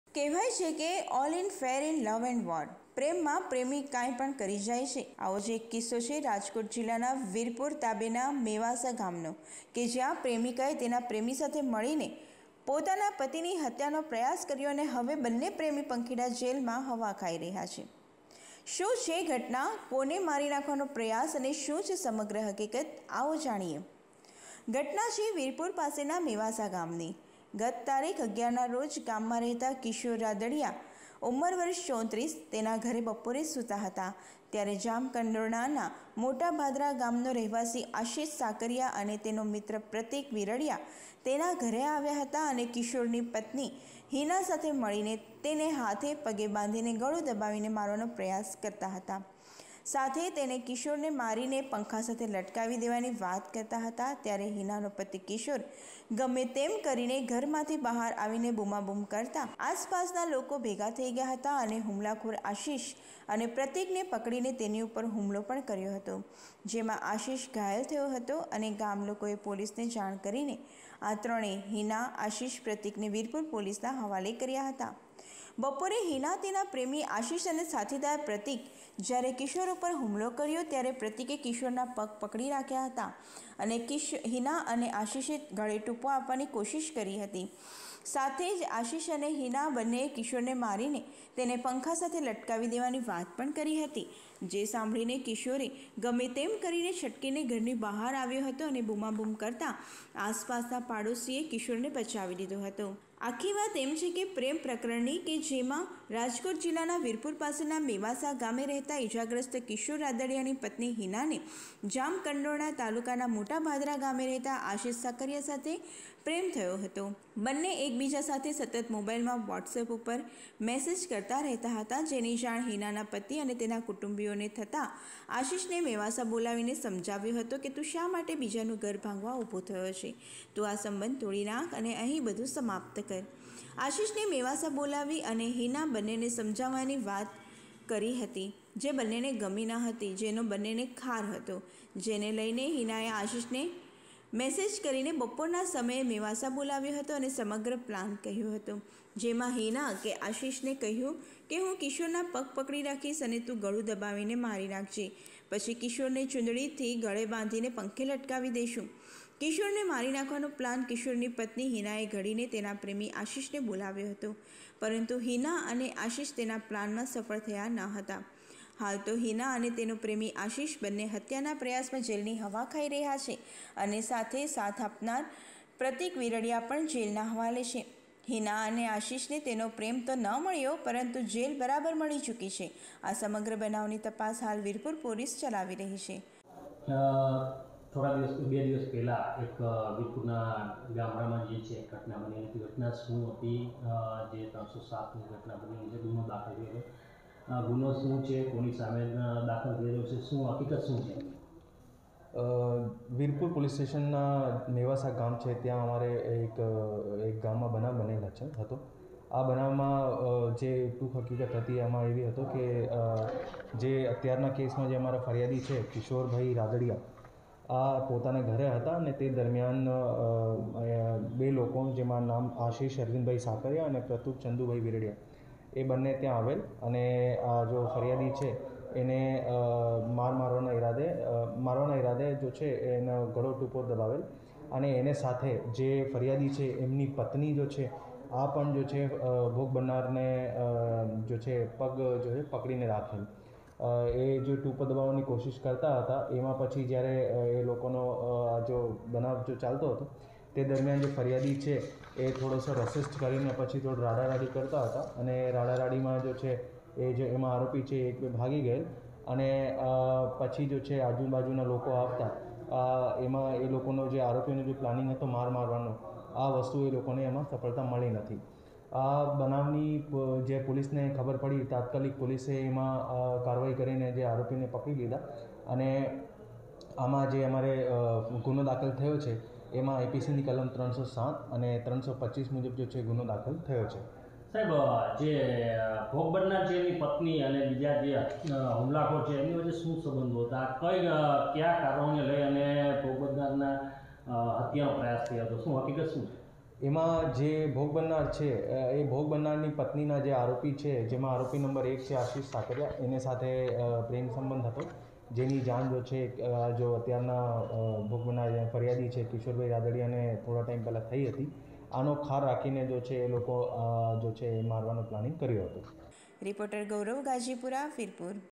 કહેવાય છે કે ALL IN FAIR IN LOVE AND WAR, પ્રેમમાં પ્રેમી કાંઈ પણ કરી જાય છે, ...|TV9INDIA GUJARATI
ઘટના છે વીરપુર પાસેના મેવાસા ગામની ગત તારીખ 11 ના રોજ ગામમાં રહેતા કિશોર ધીરુભાઈ રાદડિયા ઉ.વ. 34 તેના ઘરે બપોરે સુતા હતા ત્યારે જામકંડોરણાના મોટા ભાદરા ગામનો રહેવાસી આશિષ અરવિંદભાઈ સાકરિયા અને તેનો મિત્ર પ્રતીક વિરડીયા તેના ઘરે આવ્યા હતા અને કિશોરની પત્ની હિના સાથે મળીને તેને હાથે પગે બાંધીને ગળુ દબાવીને મારવાનો પ્રયાસ કરતા હતા અને સાથે તેને કિશોરને મારીને પંખા સાથે લટકાવી દેવાની વાત કરતા હતા, ત્યારે હિનાનો પતિ કિશોર ગમેતેમ કરીને ઘર માંથી બહાર આવીને બૂમાંબૂમ કરતા આસપાસના લોકો ભેગા થઇ ગયા હતા અને હુમલાખોર આશિષ અને પ્રતીકને પકડીને તેની ઉપર હુમલો પણ કર્યો હતો જેમાં આશિષ ઘાયલ થયેલ, ગામ લોકોએ પોલીસને જાણ કરીને આ ત્રણેય હિના,આશિષ, પ્રતિકને વીરપુર પોલીસ હવાલે કર્યા હતા,

બપોરે હિના તેના પ્રેમી આશિષ અને સાથીદાર પ્રતીકે જયારે કિશોર ઉપર હુમલો કર્યો ત્યારે પ્રતીકે કિશોરના પગ પકડી રાખેલ હતા અને હિના અને આશિષે ગળે ટૂંપો આપવાની કોશિશ કરી હતી સાથે આશિષ અને હિના બન્નેએ કિશોરને મારીને તેને પંખા સાથે લટકાવી દેવાની વાત કરી હતી, જે સાંભળીને કિશોરે ગમે તેમ કરીને છટકીને ઘરની બહાર આવ્યો હતો અને બુમાબુમ કરતા આસપાસના પાડોસીએ કિશોરને બચાવી લીધો હતો,

શા માટે હુમલો અને હત્યા નો પ્રયાસ?

આખી વાત એમ છે પ્રેમ પ્રકરણની કે જેમાં રાજકોટ જિલ્લાના વીરપુર પાસેના મેવાસા ગામે રહેતા ઈજાગ્રસ્ત કિશોર ધીરુભાઈ રાદડિયાની પત્ની હિનાને જામકંડોરણા તાલુકાના મોટા ભાદરા ગામે રહેતા આશિષ અરવિંદભાઈ સાકરિયા સાથે પ્રેમ સંબંધ થઇ ગયો હતો, અને બન્ને એકબીજા સાથે સતત મોબાઈલમાં વોટ્સએપ ઉપર મેસેજ કરતા રહેતા હતા, જેની જાણ હિનાના પતિ અને તેના કુટુંબીઓને થતા તેવોએ આશિષને મેવાસા બોલાવીને સમજાવ્યો હતો કે તું શા માટે બીજાનું ઘર ભાંગવા ઉભો થયો છો, તું આ સબંધ તોડી નાખ અને અહીં બધું સમાપ્ત કર,